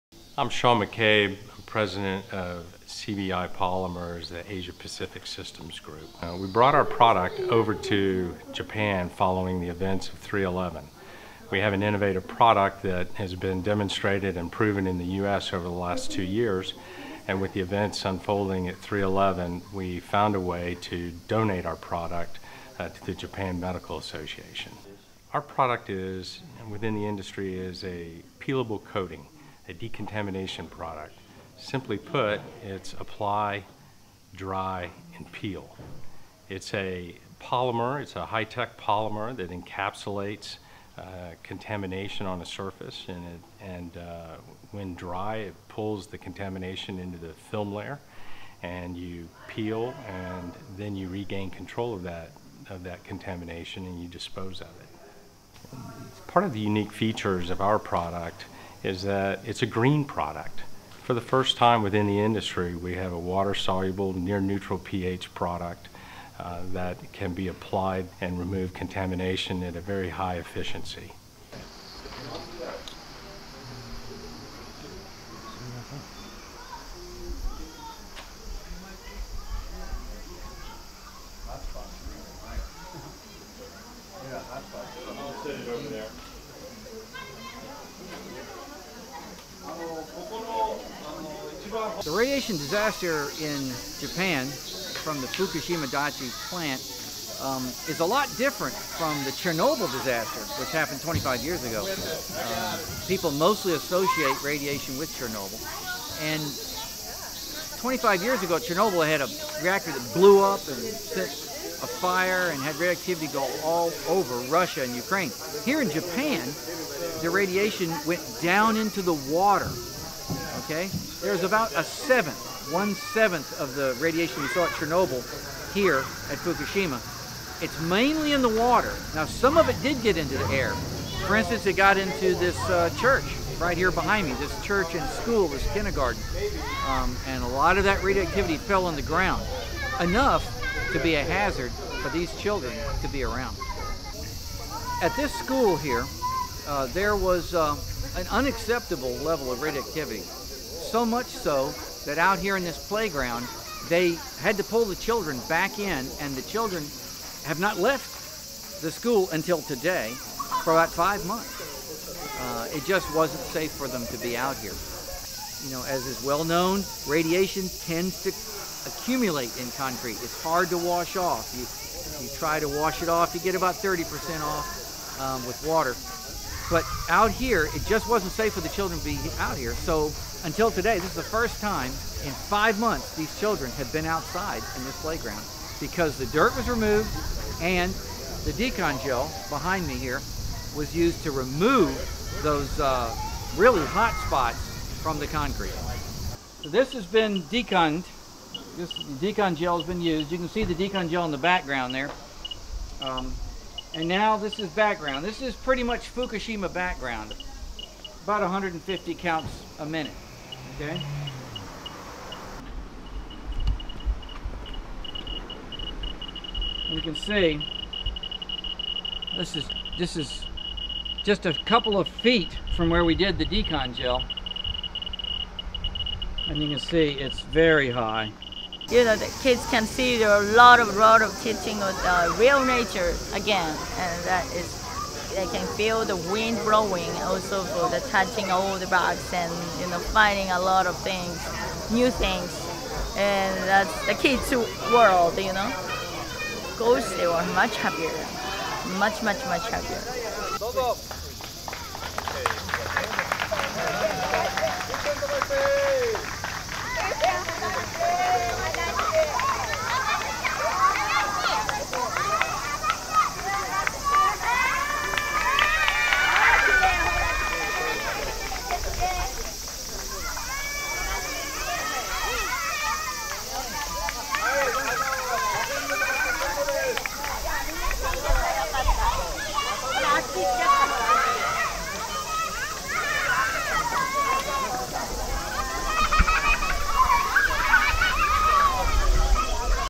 DeconGel used to clean radiation from Fukushima School
— CBI Polymers Inc., the innovator of DeconGel® nuclear decontaminant used its technology to remove radiation from the campus of the Asahimachi Baptist Church and Little Lamb Kindergarten in Fukushima, Japan, in a project CBI Polymers calls "Restore Playtime."